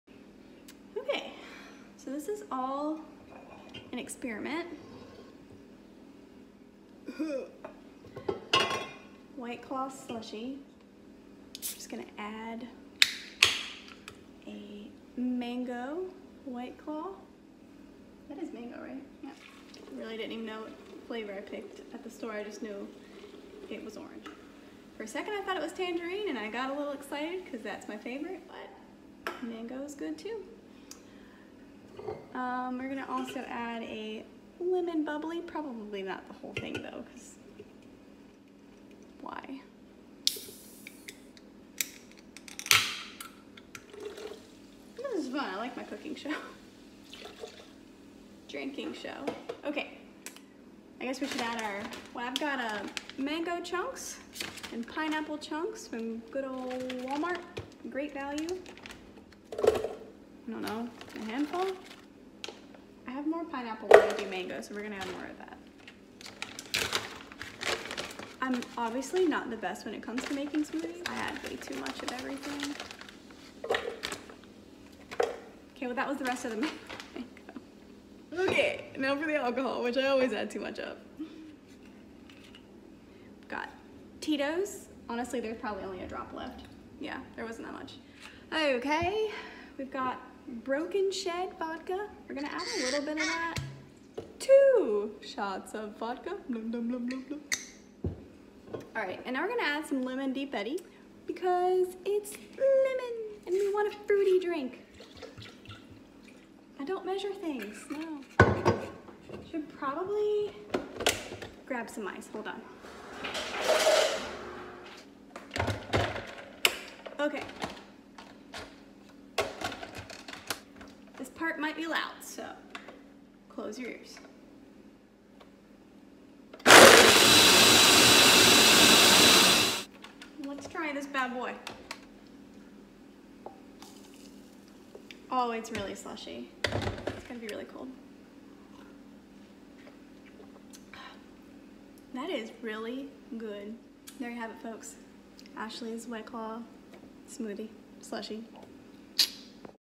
Fully loaded Mango White Claw Slushee
On this episode of my cooking (drinking show) we’re making mango white claw slushees. It’s fairly simple:  One white claw flavor of your choice, basically any frozen fruit you may have around, vodka of your choice (fruit flavored if you’re feeling fancy.) I added a lemon carbonated drink which is probably not all that necessary. But you do you booboo.